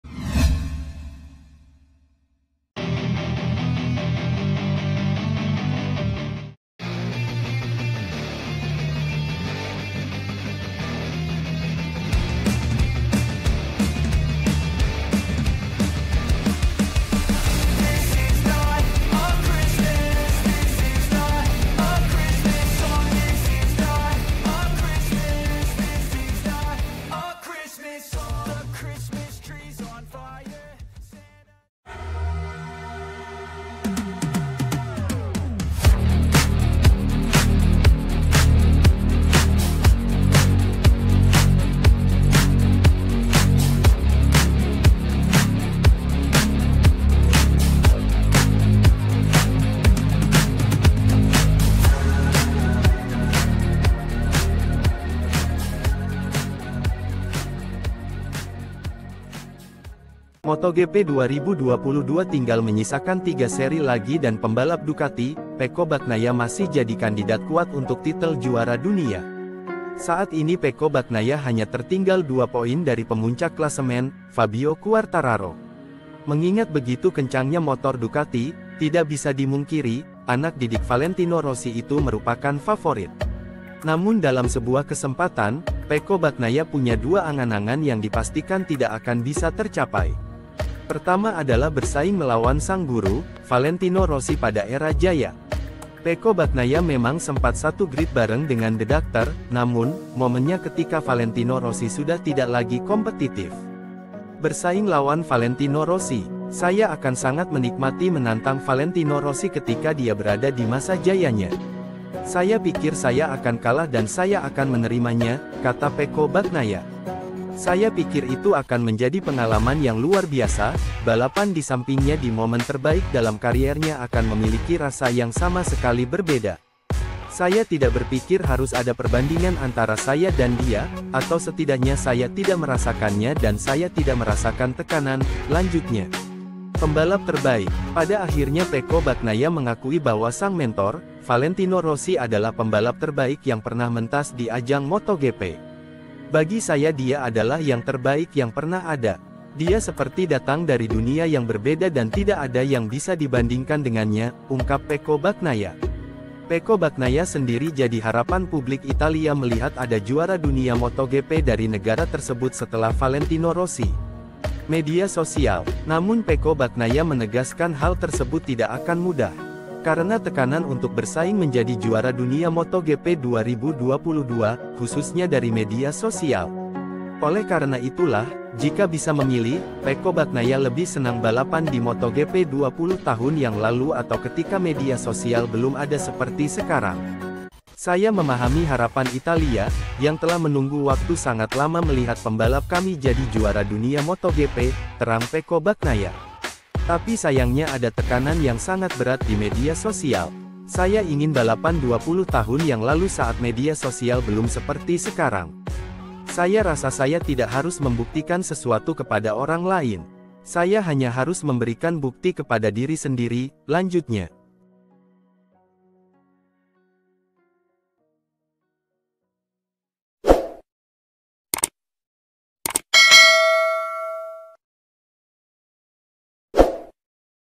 Motogp| Pecco Bagnaia Ingin Menantang Valentino Rossi Pada Era Jayanya⁉️Hindari Tekanan Medsos ‼️
Dalam sebuah kesempatan, Pecco Bagnaia punya dua angan-angan yang dipastikan tidak akan bisa tercapai.

Pertama adalah bersaing melawan sang guru, Valentino Rossi pada era jaya. Pecco Bagnaia memang sempat satu grid bareng The Doctor, namun momennya ketika Valentino Rossi sudah tidak lagi kompetitif.

Namun Pecco Bagnaia menegaskan hal tersebut tidak akan mudah.